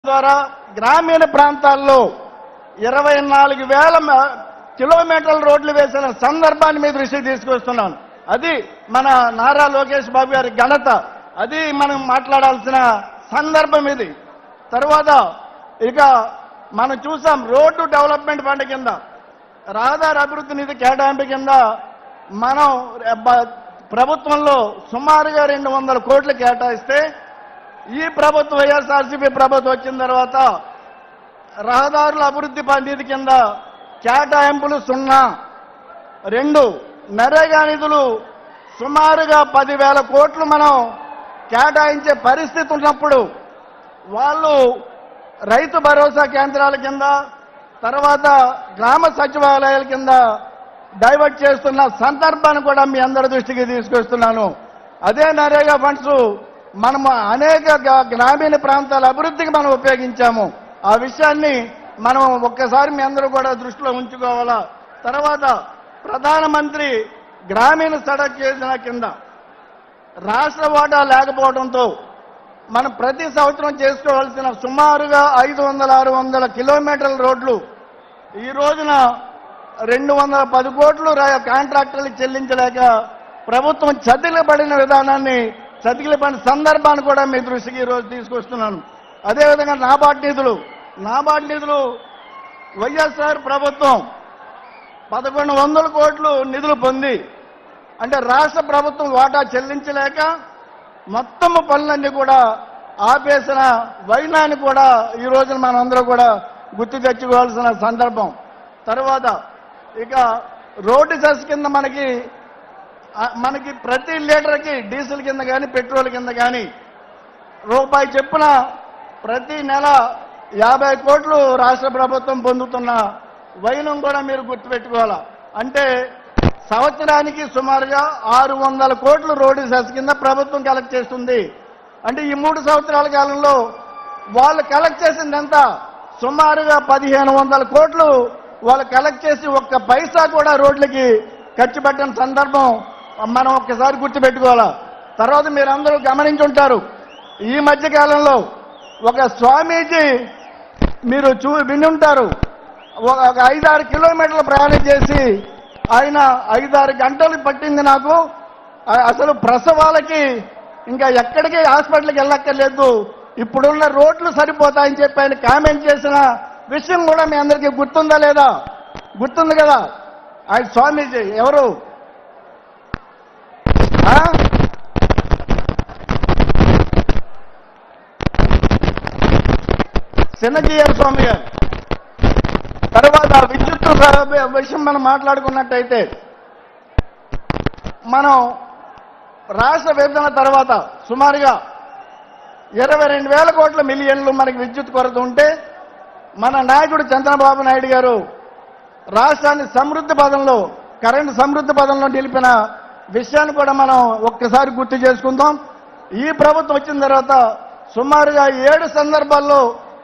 ఇప్పుడున్న ప్రభుత్వం వాళ్ళ ప్రసవం కోసం హాస్పిటల్ కి వెళ్లాల్సిన పని లేదు రోడ్డు మీదే.. || ABN Telugu
LIVE : మహానాడు.. పసుపు పండుగ || TDP Mahanadu 2022 LIVE || ABN  LIVE